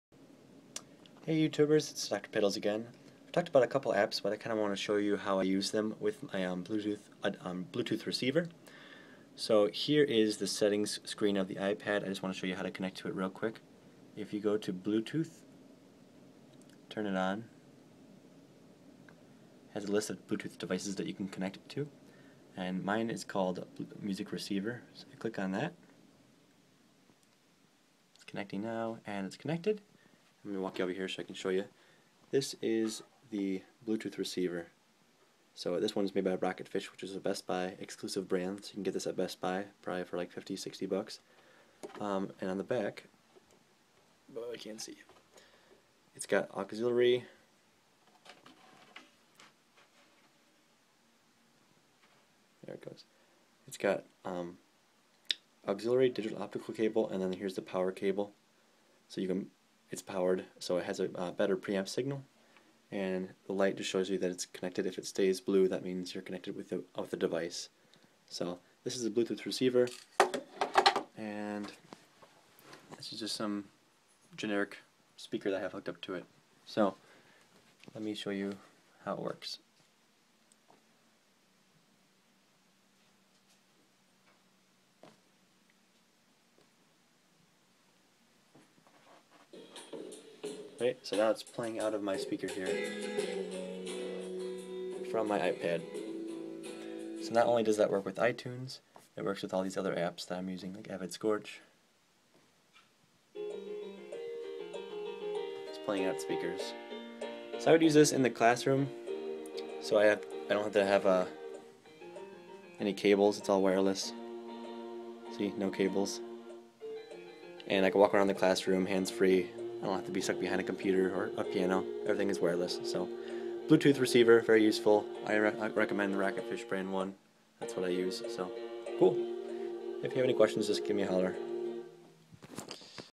Bluetooth receiver
I use a rocketfish Bluetooth receiver to send all the audio from my iPad wirelessly to a set of larger speakers.